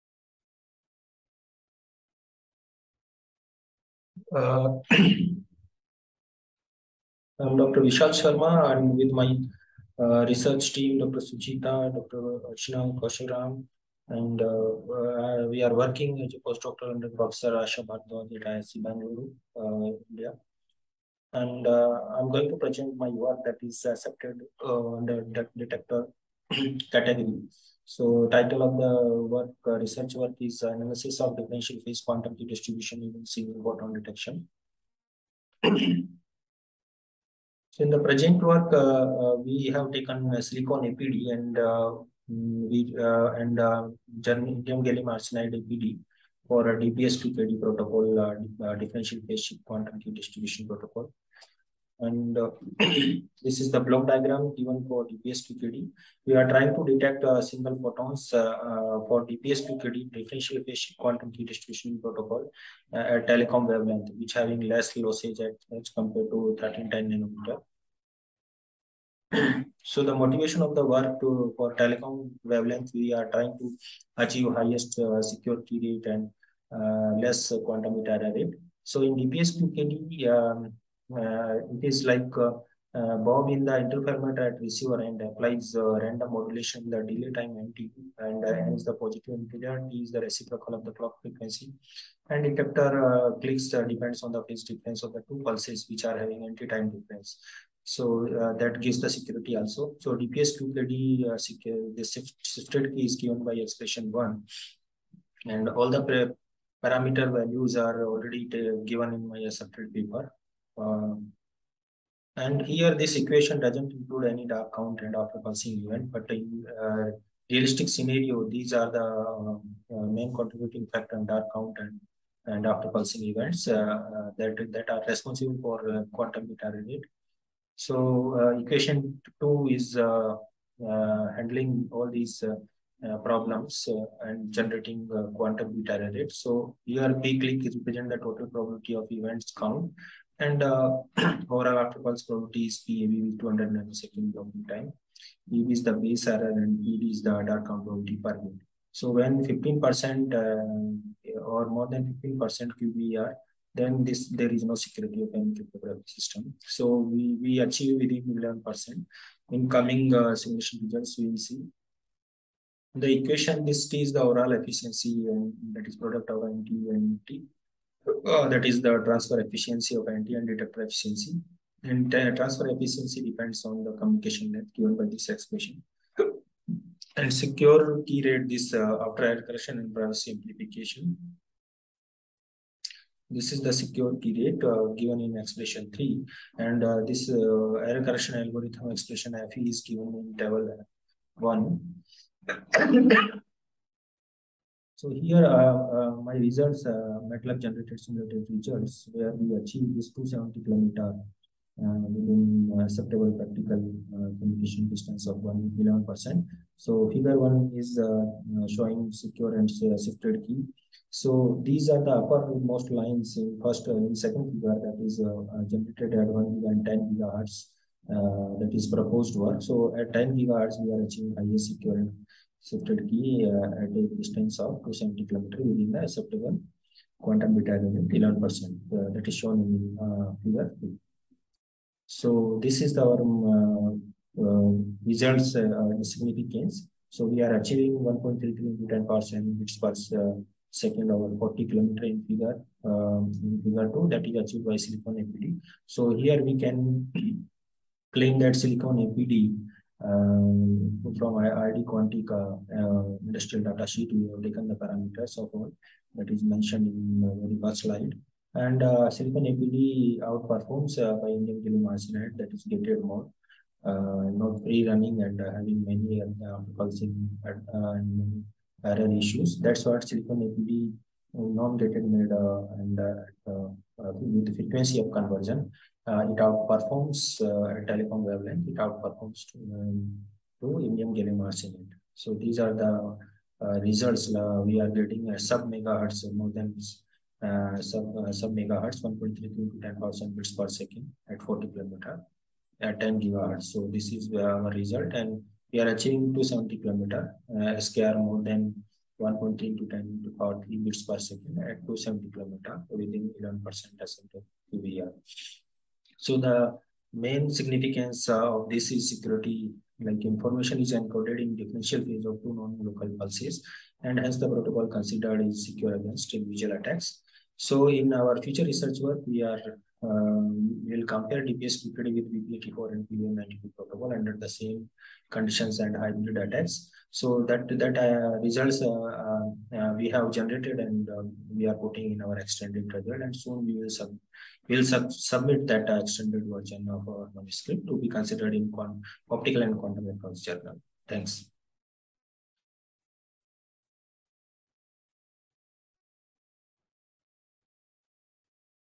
22D09 Analysis of Differential Phase Shift Quantum Key Distribution using single-photon detectors
We investigated the performance of differential phase shift quantum key distribution using InGaAs/InP and Silicon-APD (avalanche photo diode) for generating secure keys, secure communication distance, and bit error rates under the various operating conditions.We compare the quantum bit error rate and the secure key generation rate as a function of communication length.Our simulation results show that with frequency conversion at telecommunication wavelength the silicon-APD contributes in enhanced communication rates and higher communication distances than InGaAs/InP APD for optical fiber-based quantum key distribution applications.